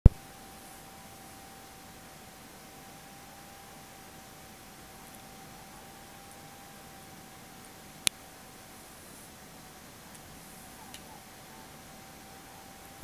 白目で寝るシュナウザー
生後二か月のシュナウザー。白目で寝ています。